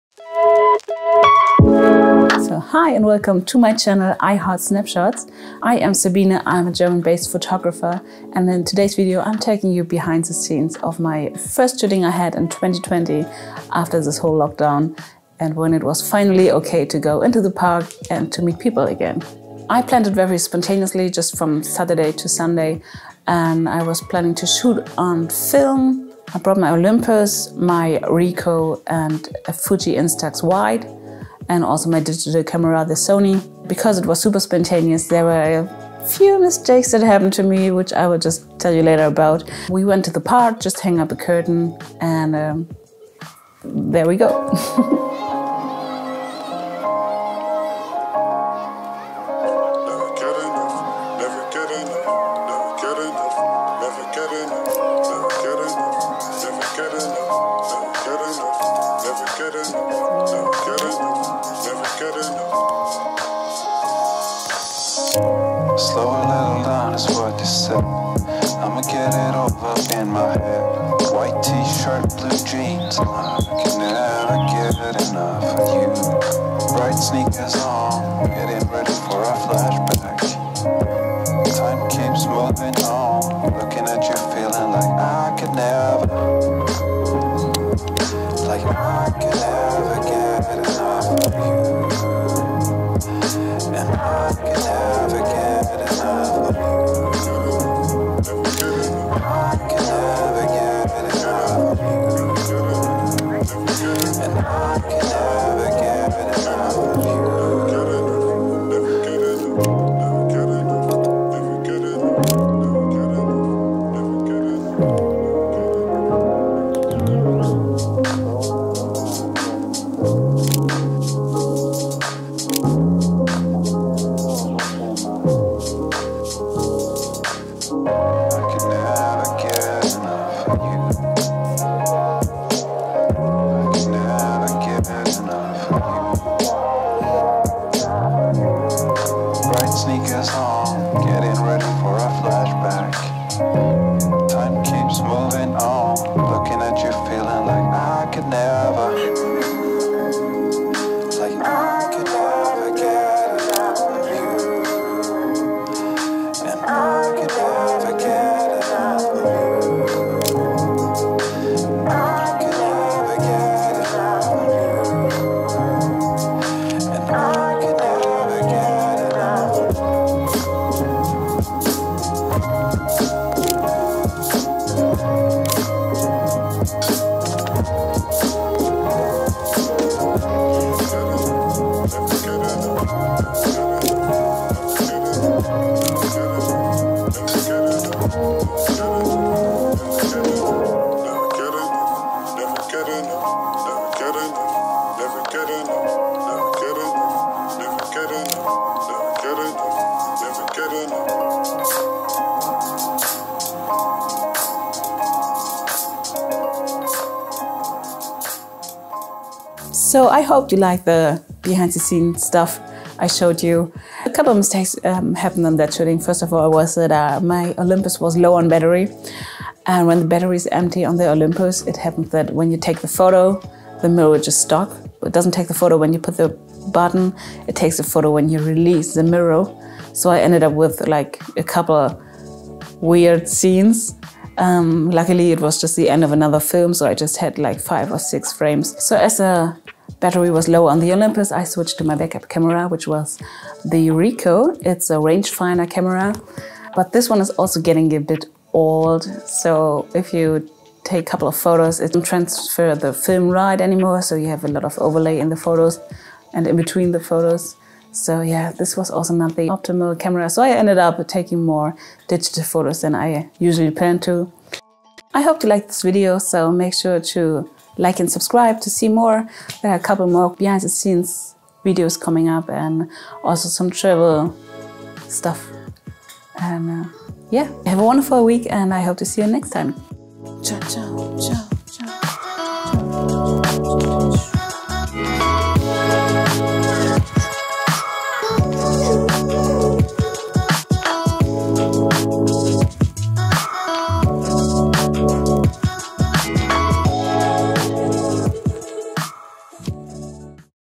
Shooting Portrait on Film & Digital - [Olympus OM-2N / Ricoh 35 ZF / Fuji Instax Wide / Sony a7III]
In my first video I'm taking you behind the scenes of my first portrait shoot in 2020. I shot on film with my Olympus OM 2N, Ricoh 35 ZF and Fuji Instax Wide. For some digital portrait shots I used my Sony a7III.
I forgot to mention that I used Kodak Gold and expired drugstore film (dm).